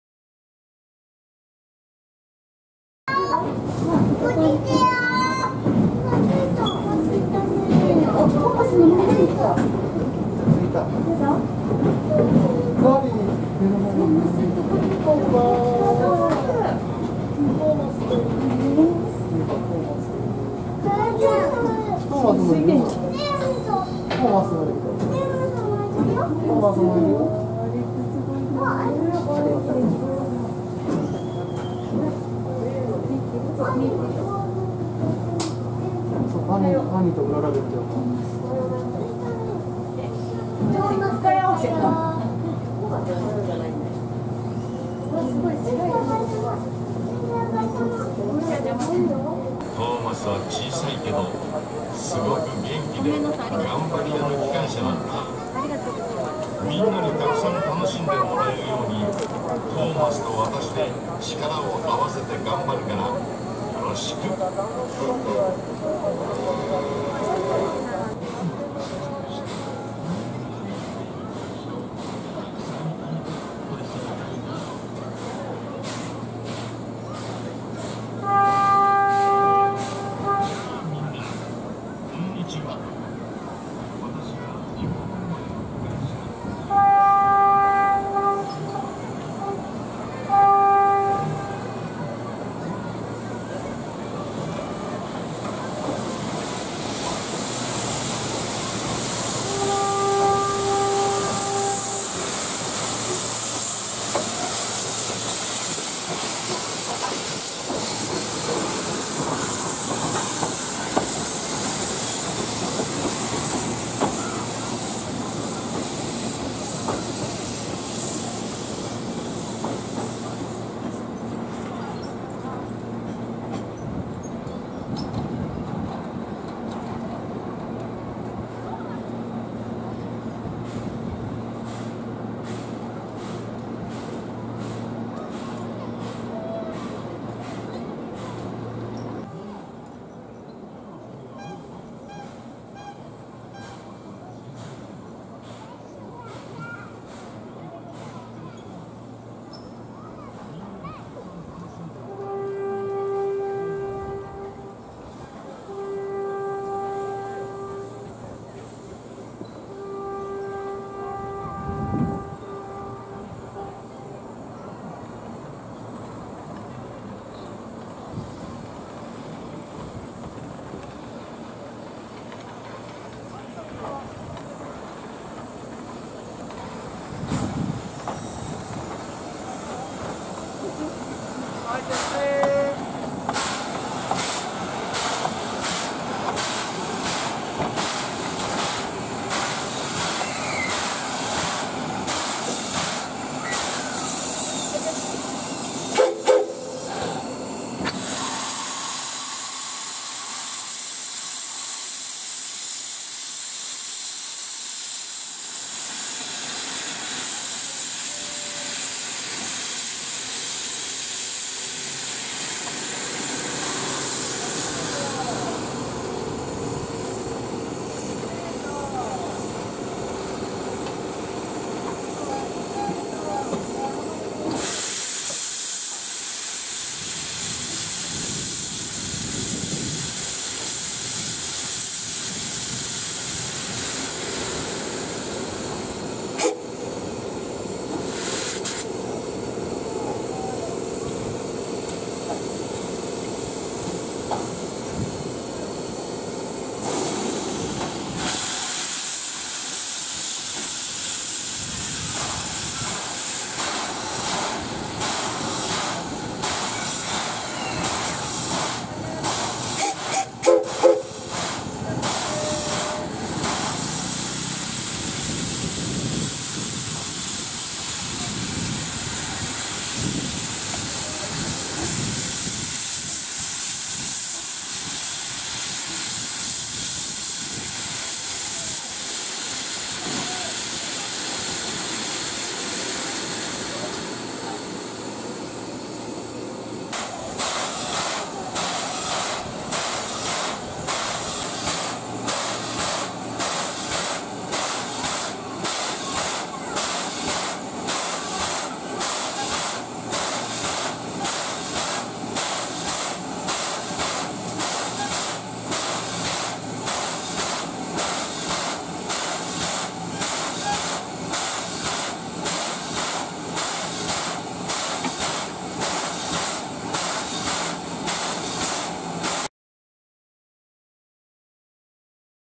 大鉄トーマス、今シーズン最後の４並び＆入換（2021・9・27）
9/27、今シーズン最後の千頭駅でのトーマスキャラ４並びと入換を撮りました。
大鉄のトーマスは12月24日(金)～2022(令和4)年1月3日(月)、1月7日(金)～1月10日(月)にクリスマス＆お正月で運行が決定しています。